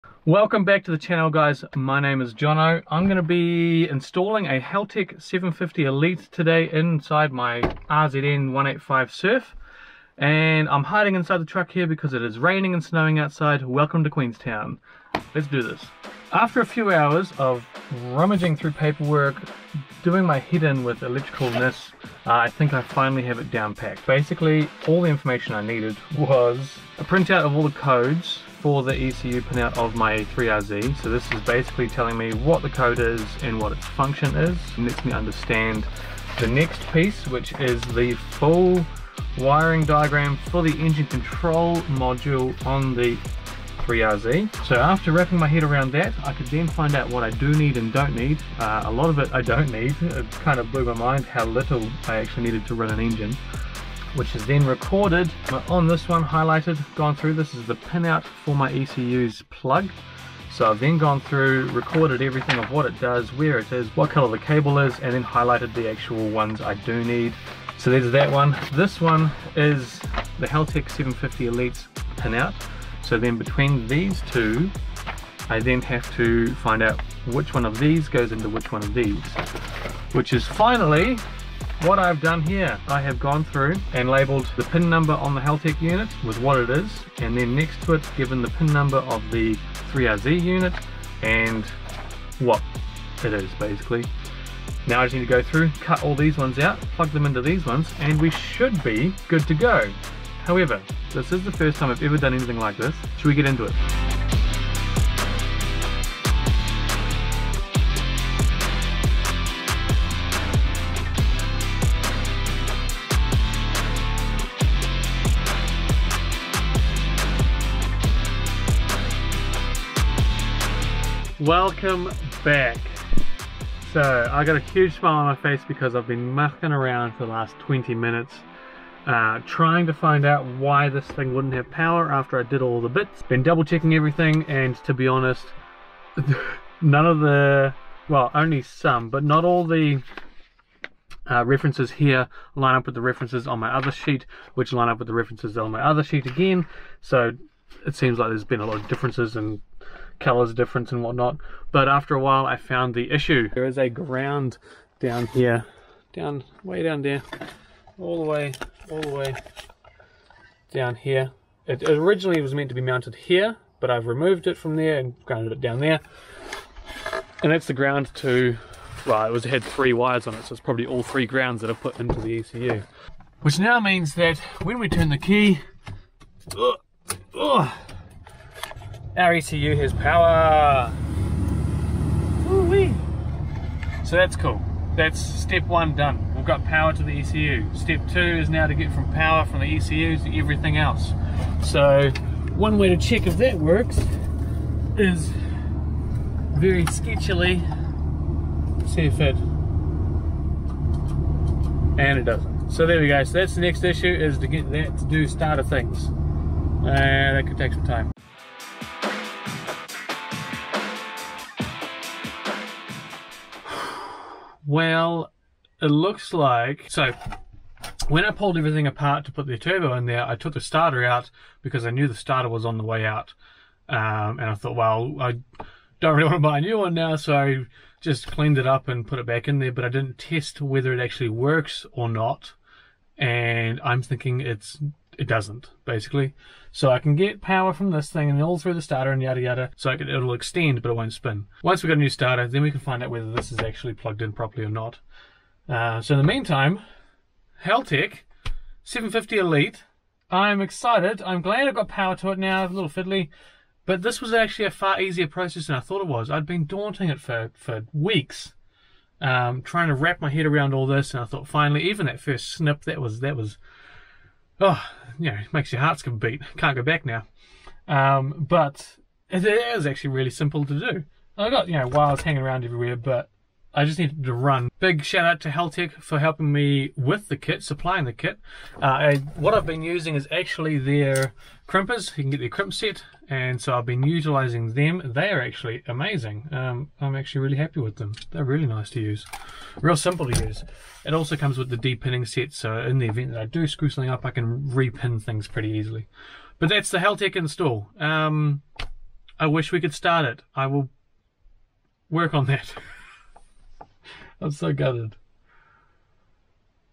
Haltech 750 elite install to 3RZ Turbo
I quickly run down the process I used to install and wire in the Haltech 750 elite into my Turbo 3RZ-FE Hilux Surf. If needed, I can write up the breakdown of what wires go where to help others in the future.